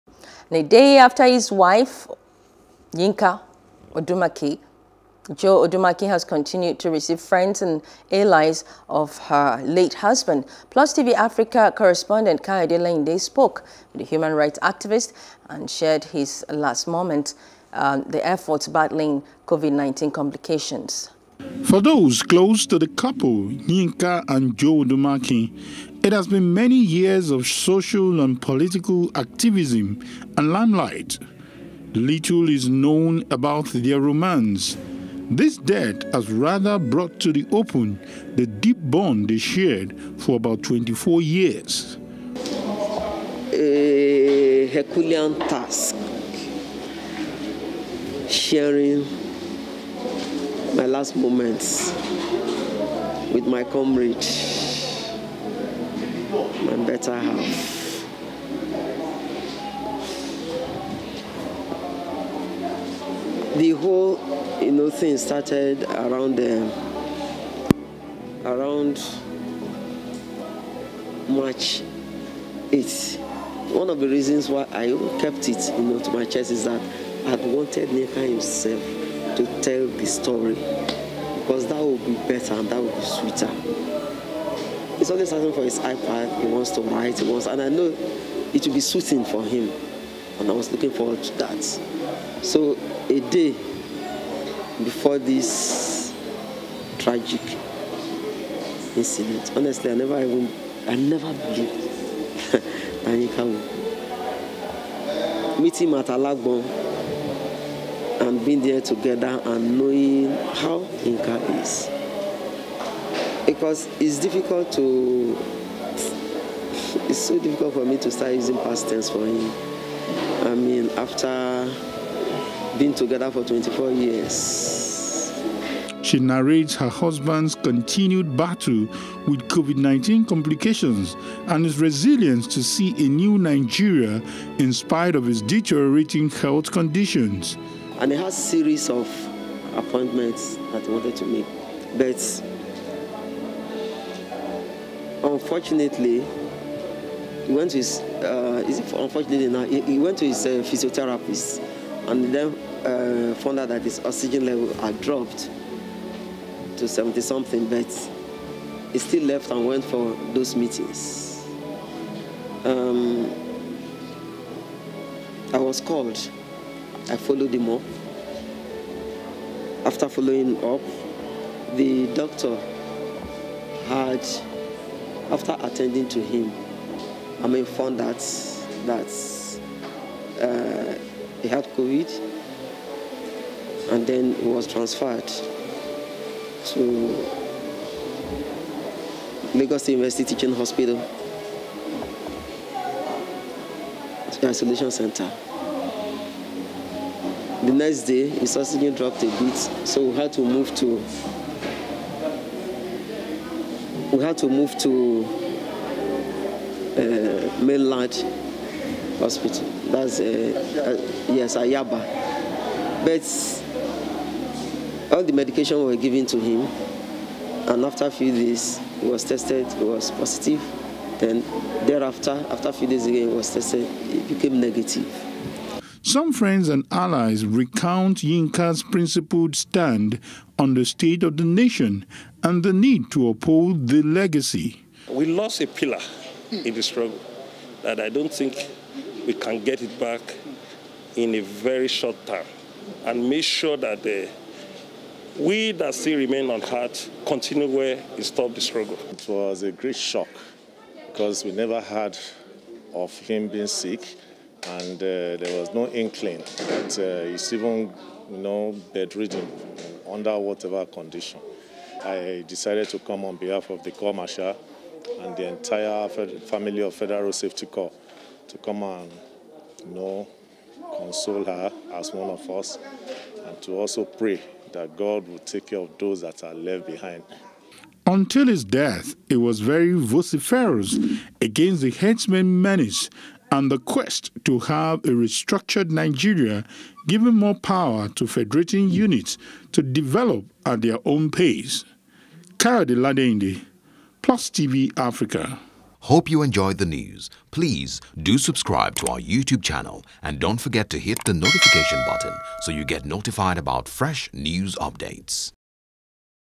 Yinka Odumakin's Wife Recounts His Last Moment As Mourners Continue To Pay Tributes | NEWS
A day after his death, wife of Yinka Odumakin, Joe has continued to receive friends and allies of her late husband.

PlusTv Africa correspondent, Kayode Ladeinde spoke with the human rights activist and she shared his last moments and the effort battling covid-19 complications.